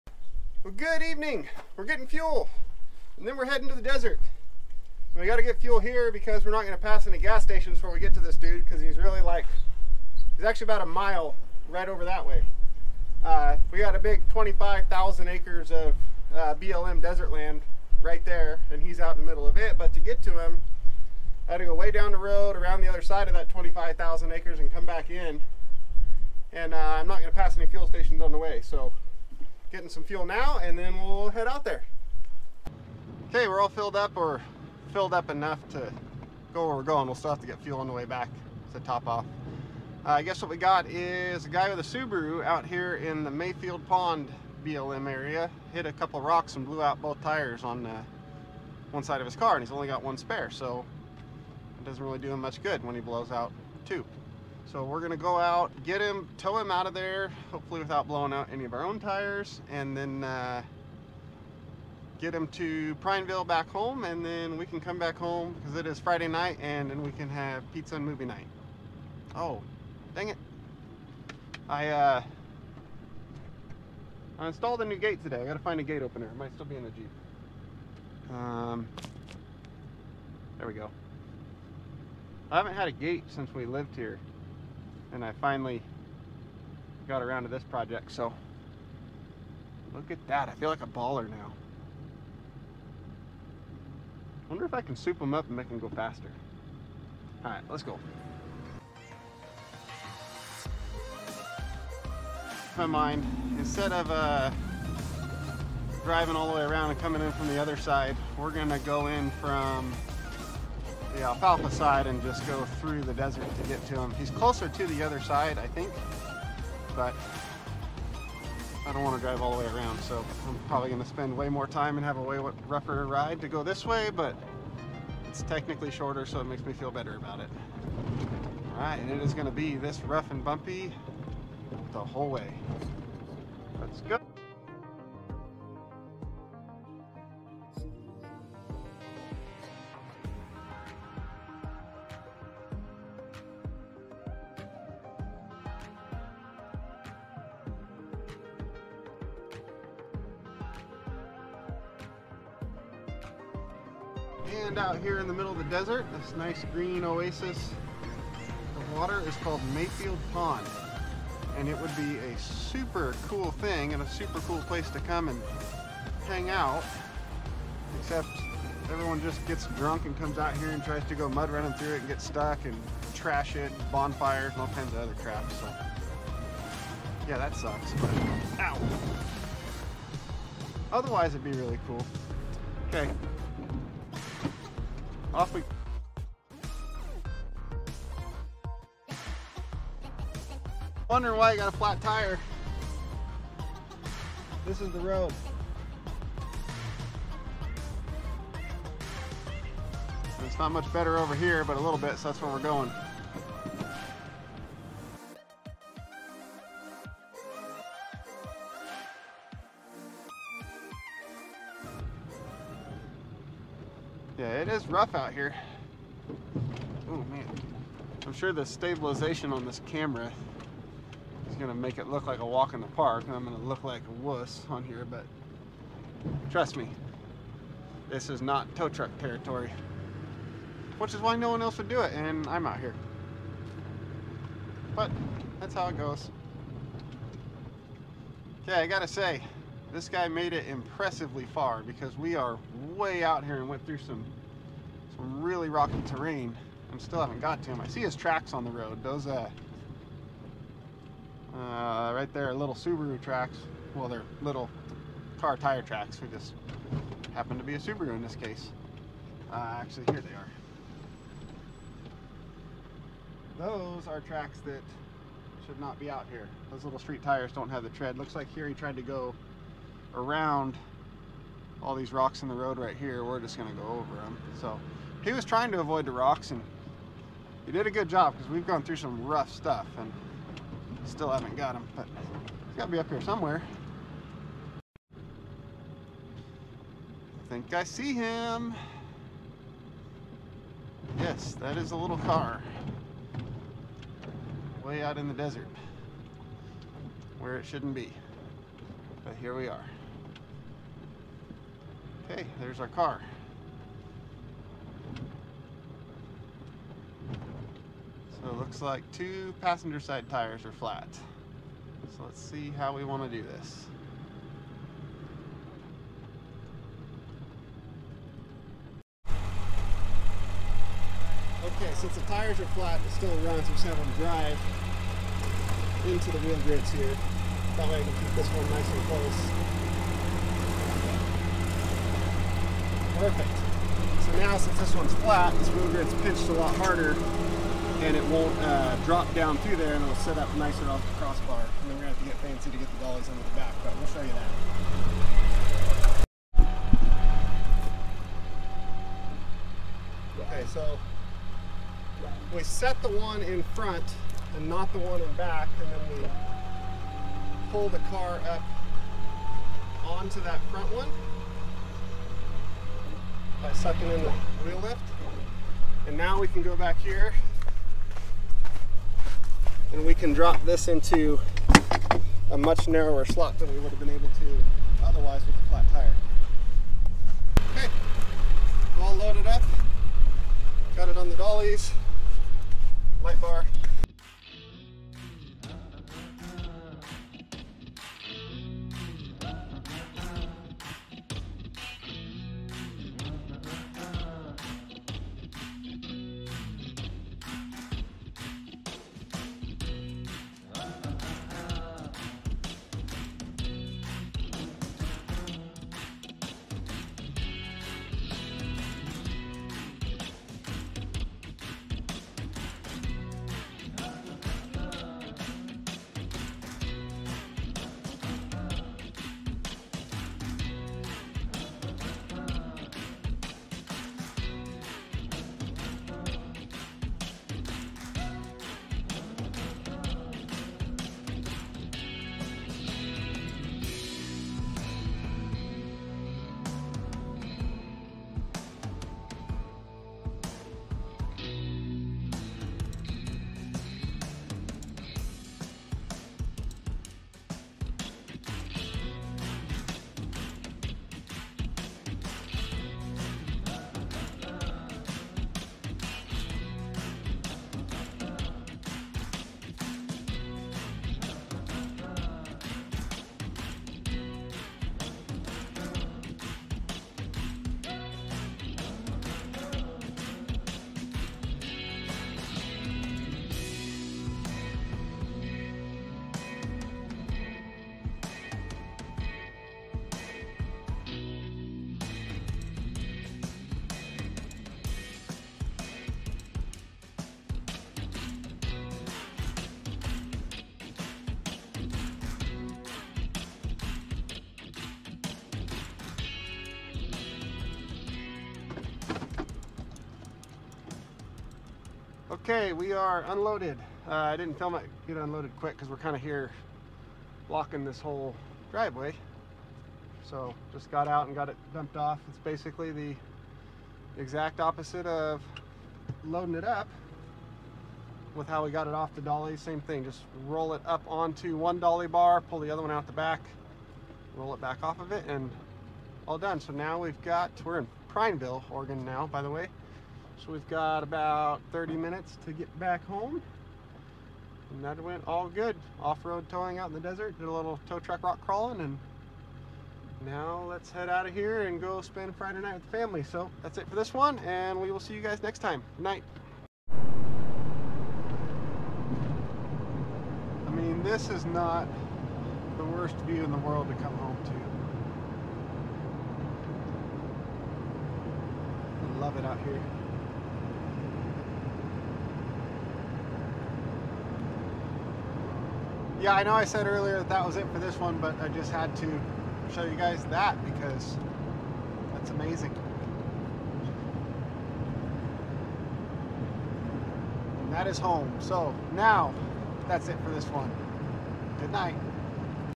Off roading in a tow truck! Little car stuck in the desert!
A Subaru Legacy tried to take the scenic route through the desert and ended up slicing two tires on a rock.  Since he only had one spare... that meant a call for a tow truck!  I took the 4x4 wrecker out to load it up on the Collins dollies and get him towed back home